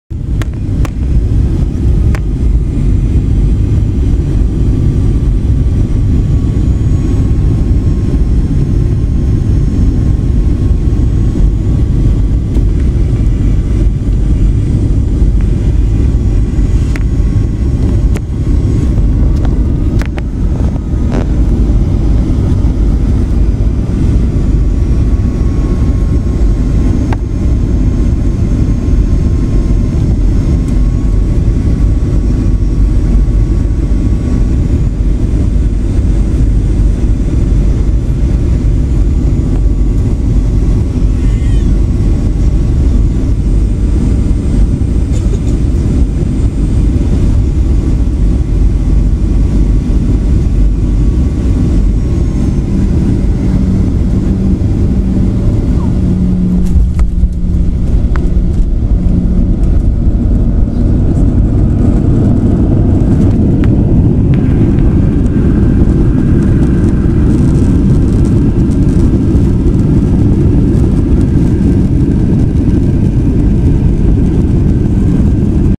Westjet 737 landing in Calgary
Landing in Calgary, flight from Kelowna. Westjet 737, recorded with a blackberry playbook.r